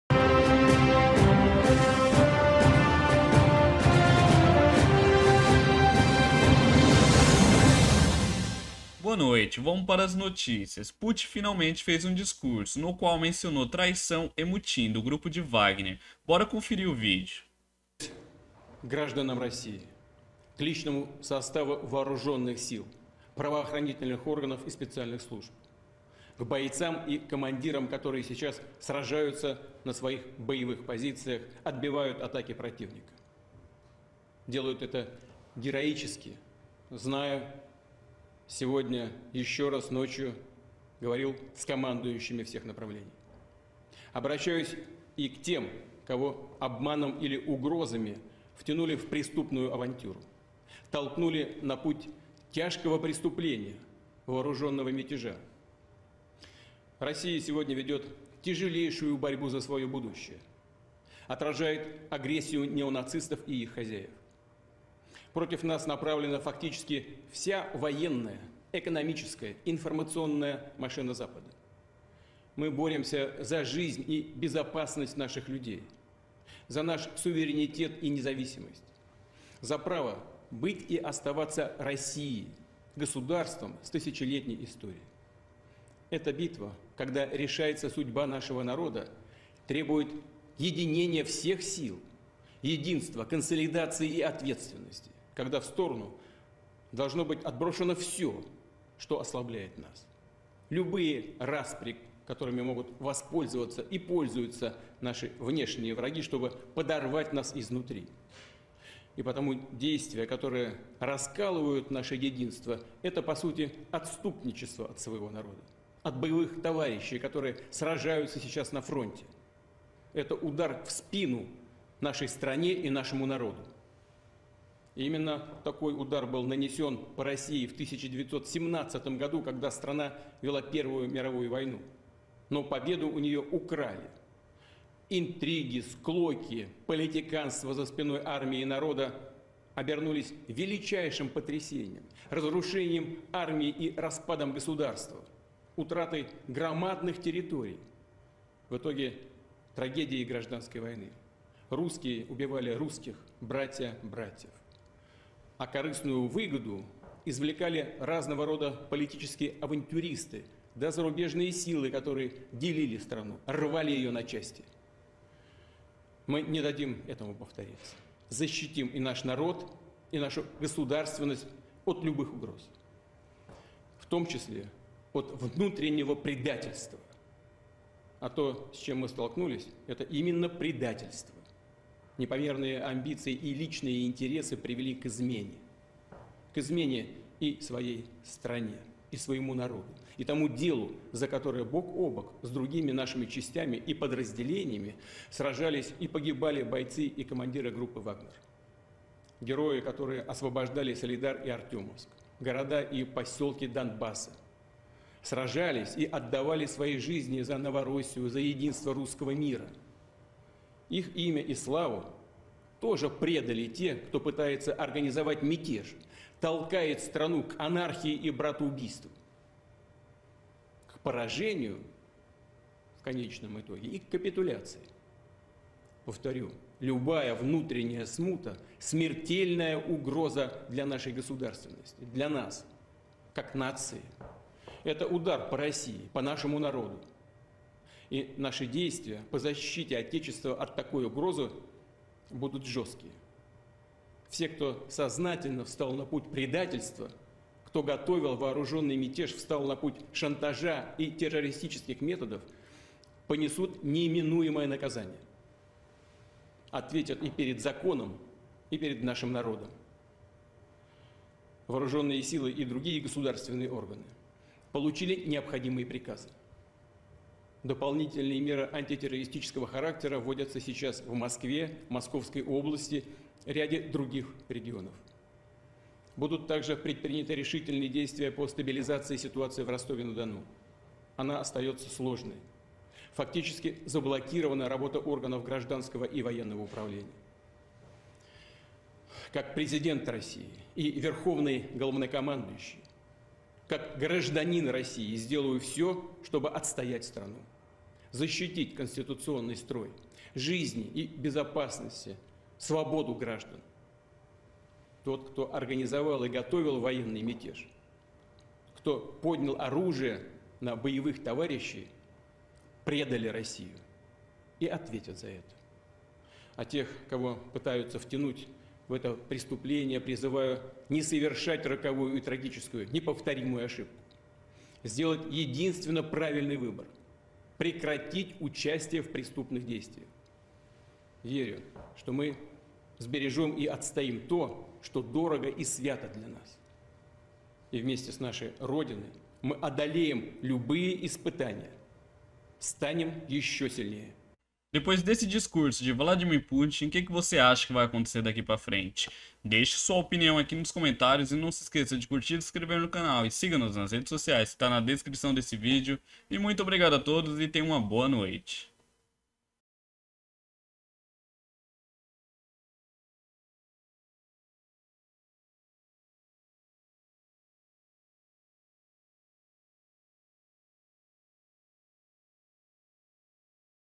PUTIN FINALMENTE FAZ UM DISCURSO NO QUAL MENCIONOU "TRAIÇÃO"
Bem Vindo ao Canal Do Jhony